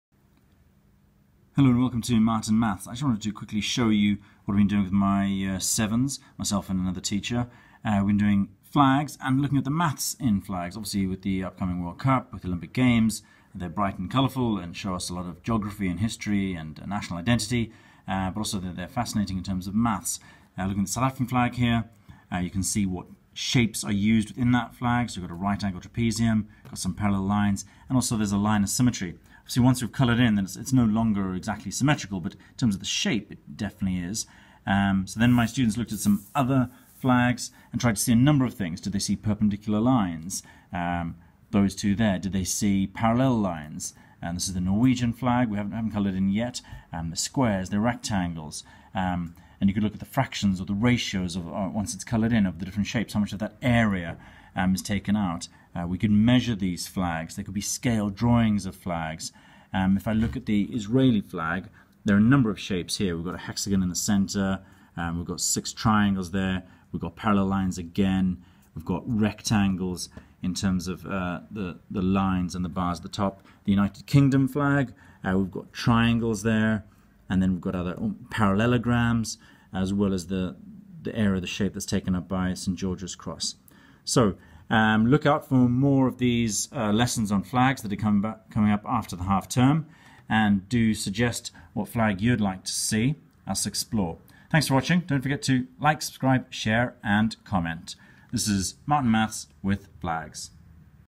Maths in flags
Flags and their shapes
and mathematical concepts related to flags

South African flag
UK flag
Israeli flag
Jamaican flag
Norwegian flag

Which is your favourite flag? whats your home country flag?

Lines of symmetry
Shapes like trapezium, rectangle, triangles,
Parallel and perpendicular lines
Area and perimeter measurement

Send in your own flag!

and look out for more Flag maths on the channel!

Short

Maths in flags! Shapes, ratio, fractions and parallel lines